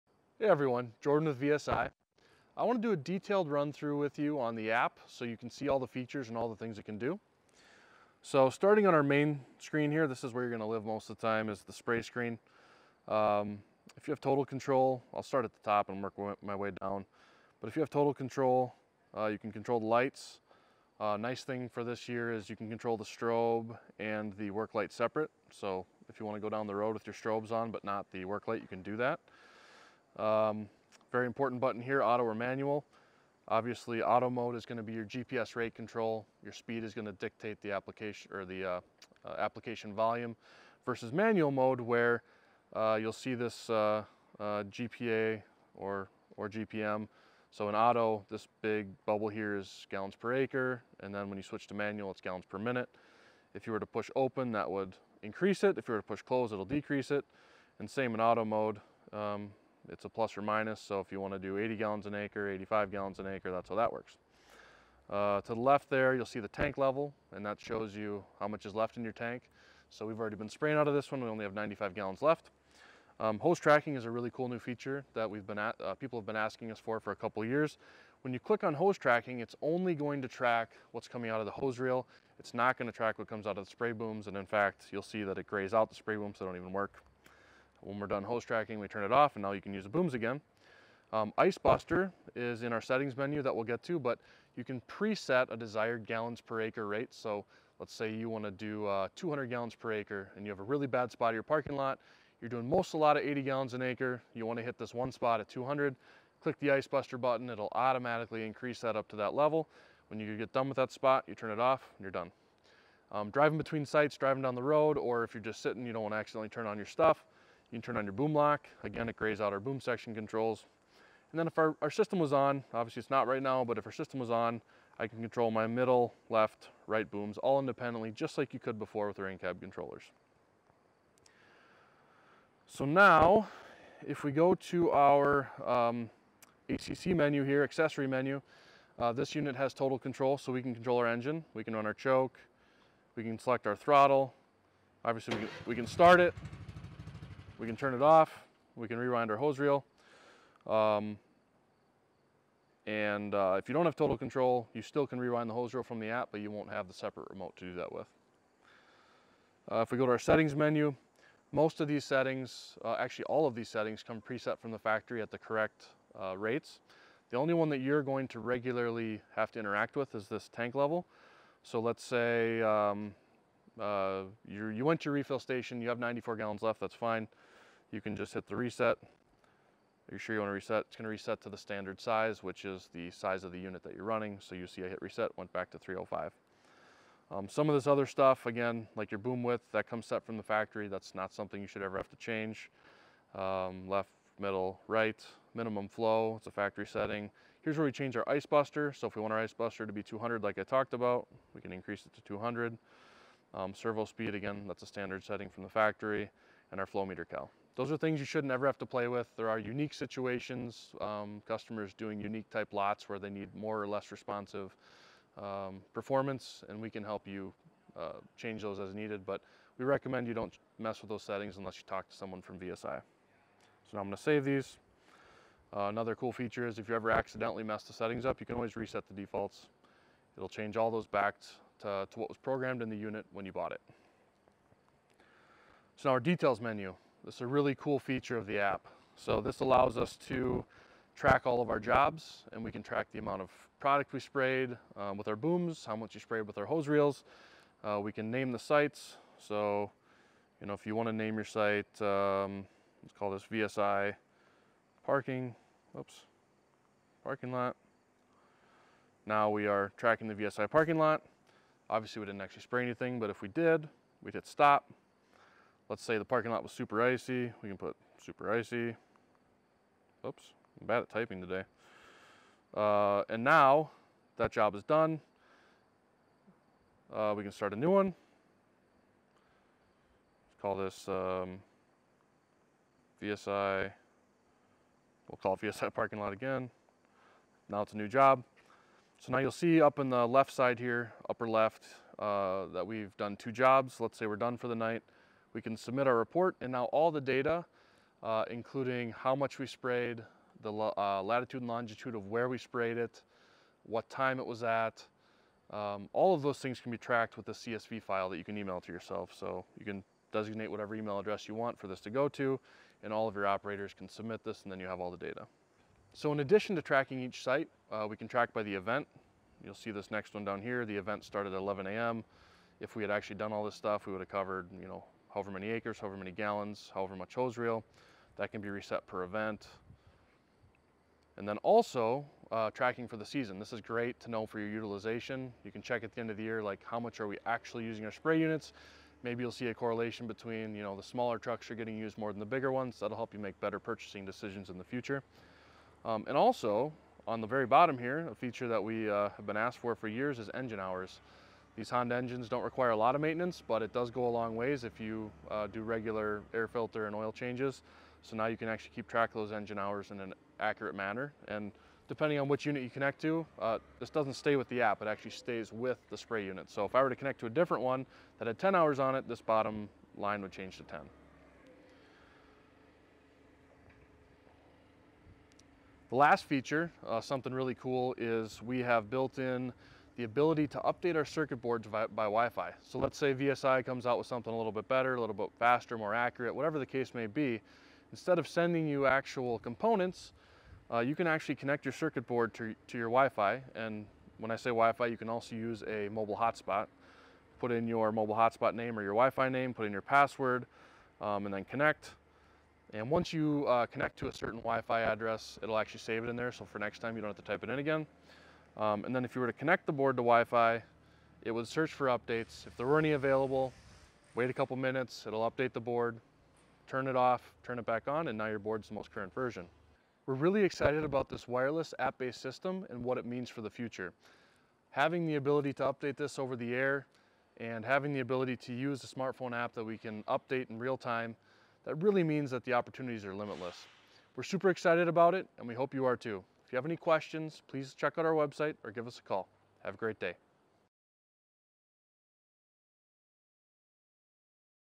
2020 VSI Spray Control App Overview
This video is a more in depth overview of the VSI Spray Control App.  Feedback is encouraged as we now have a way to update features remotely and continue adding value to your purchase of a VSI unit.